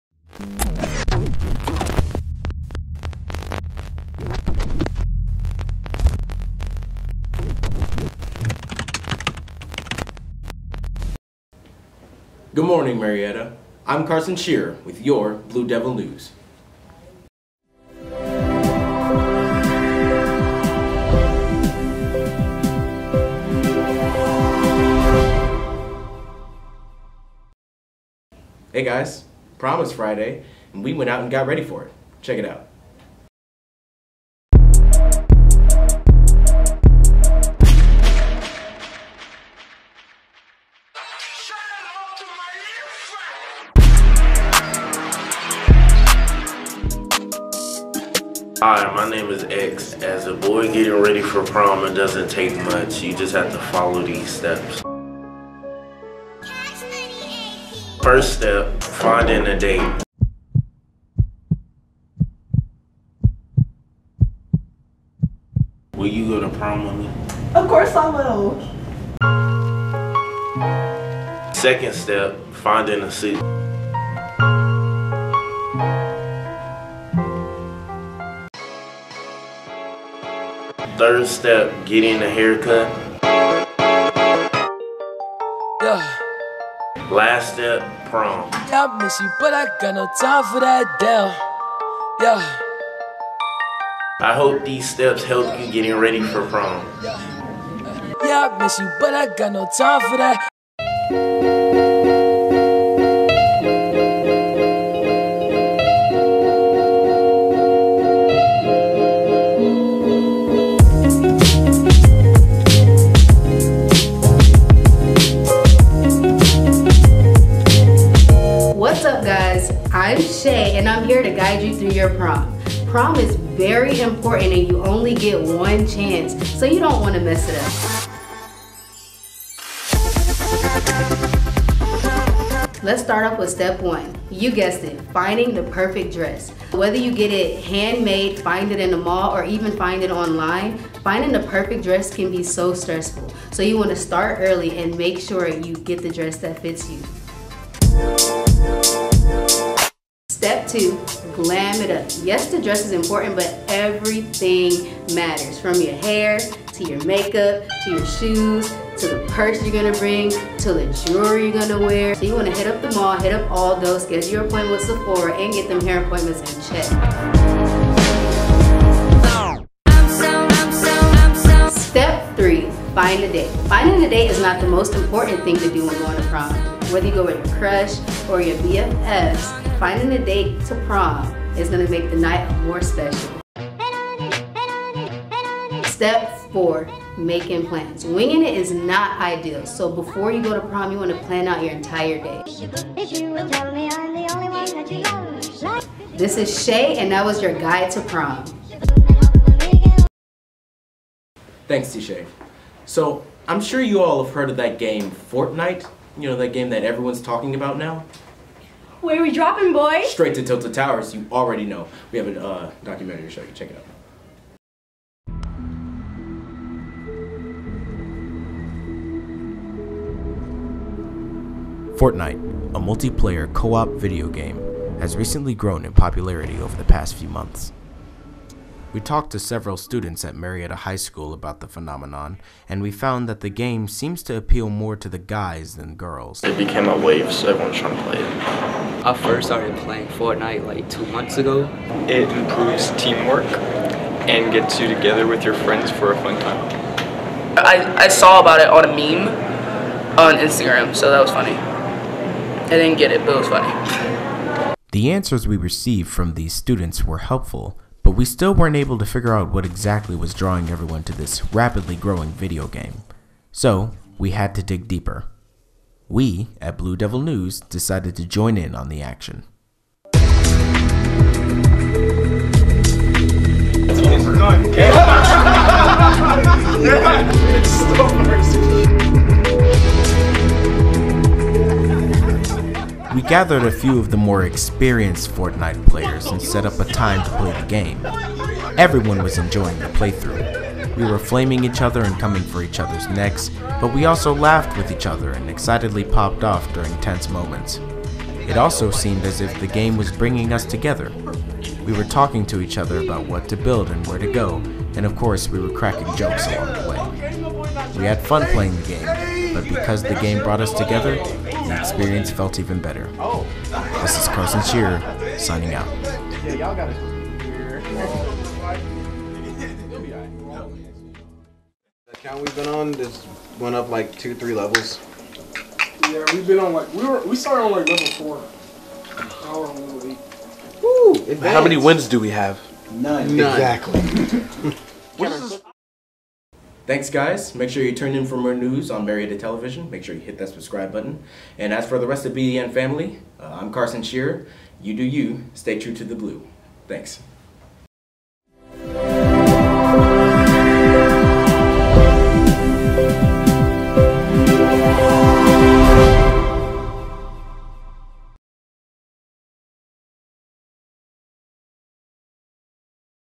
Blue Devil News - April 10, 2018
New anchor Carson Shearer dishes on prom preparedness and that whole Fortnite phenomenon that's got kids going crazy!